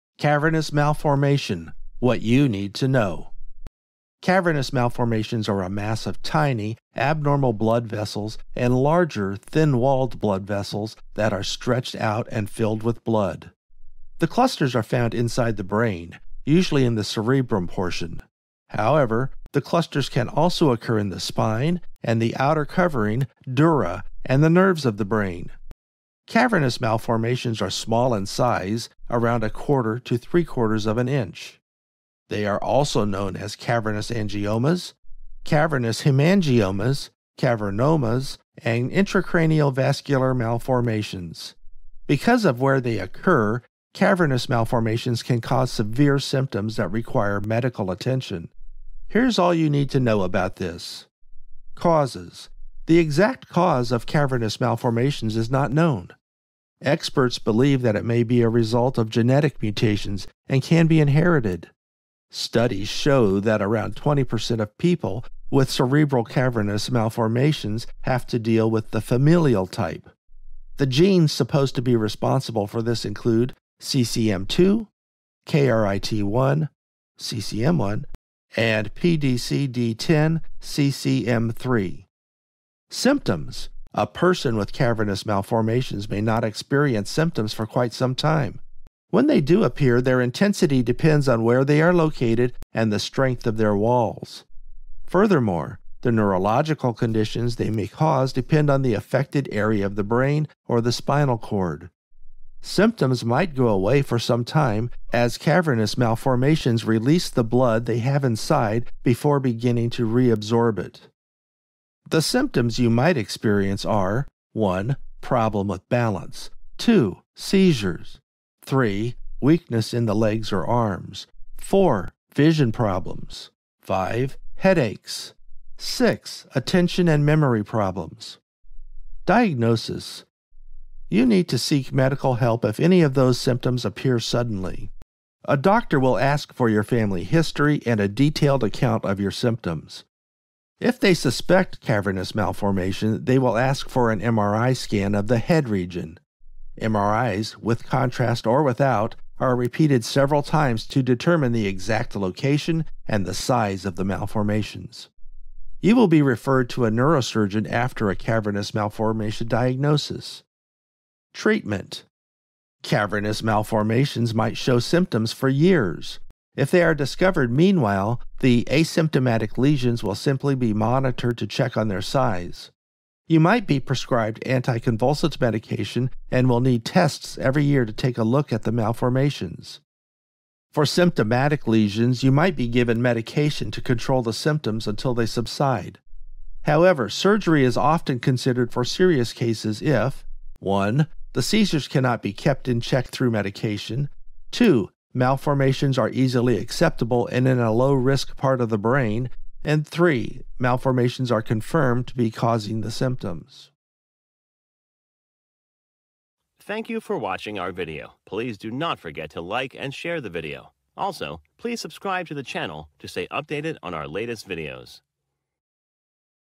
Cavernous Malformations: What You Need to Know
Chapters

0:00 Introduction
0:52 Causes of Cavernous Malformations
1:23 Symptoms of Cavernous Malformations
2:09 Diagnosis of Cavernous Malformations
2:41 Treatment of Cavernous Malformations

• Cavernous malformations are a mass of tiny, abnormal blood vessels and larger, thin-walled blood vessels that are stretched out and filled with blood.
 • The clusters are found inside the brain, usually in the cerebrum portion.
 • However, the clusters can also occur in the spine and the outer covering (dura), and nerves of the brain.
 • Cavernous malformations are small in size, around a quarter to three-quarters of an inch.
 • They are also known as cavernous angiomas, cavernous hemangiomas, cavernomas, and intracranial vascular malformations.
 • Because of where they occur, cavernous malformations can cause severe symptoms that require medical attention.
 • Here’s all you need to know about this:
Causes
 • The exact cause of cavernous malformations is not known.
 • Experts believe that it may be a result of genetic mutations and can be inherited.
 • Studies show that around 20% of people with cerebral cavernous malformations have to deal with the familial type.
 • The genes supposed to be responsible for this include CCM2, KRIT1 (CCM1), and PDCD10 (CCM3).
Symptoms
 • A person with cavernous malformations may not experience symptoms for quite some time.
 • When they do appear, their intensity depends on where they are located and the strength of their walls.
 • Furthermore, the neurological conditions they may cause depend on the affected area of the brain or the spinal cord.
 • Symptoms might go away for some time as cavernous malformations release the blood they have inside before beginning to reabsorb it.
 • The symptoms you might experience are:
 • Problems with balance
 • Seizures
 • Weakness in the legs or arms
 • Vision problems
 • Headaches
 • Attention and memory problems
Diagnosis
 • You need to seek medical help if any of those symptoms appear suddenly.
 • A doctor will ask for your family history and a detailed account of your symptoms.
 • If they suspect cavernous malformation, they will ask for an MRI scan of the head region.
 • MRIs, with contrast or without, are repeated several times to determine the exact location and the size of the malformations.
 • You will be referred to a neurosurgeon after a cavernous malformation diagnosis.
Treatment
 • Cavernous malformations might show symptoms for years.
 • If they are discovered meanwhile, the “asymptomatic lesions” will simply be monitored to check on their size.
 • You might be prescribed anticonvulsant medication and will need tests every year to take a look at the malformations.
 • For symptomatic lesions, you might be given medication to control the symptoms until they subside.
 • However, surgery is often considered for serious cases if:
 • The seizures cannot be kept in check through medication
 • The malformations are easily accessible and in a low-risk part of the brain
 • The malformations are confirmed to be causing the symptoms